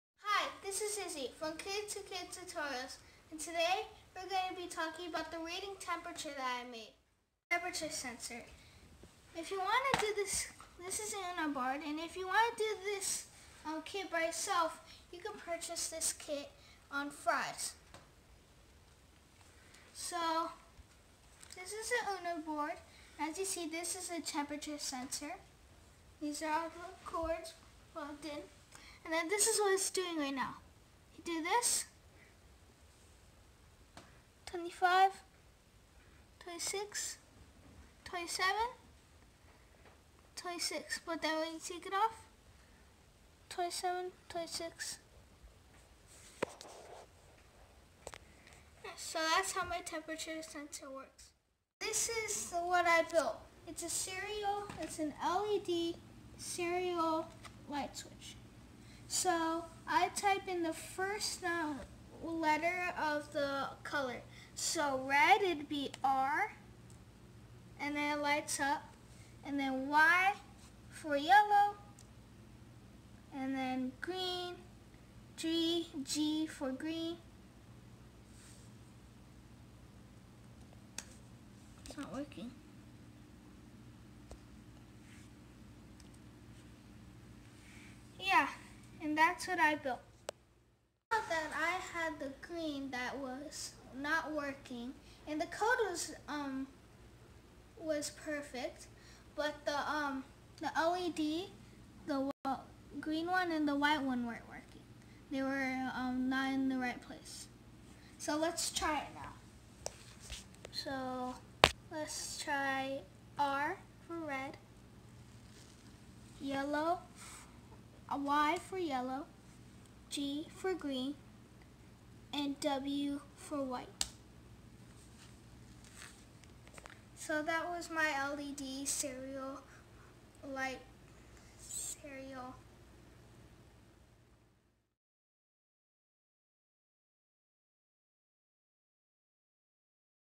Arduino Temp Sensor and LED Switch
Issy shares his Arduino temp sensor and LED switch.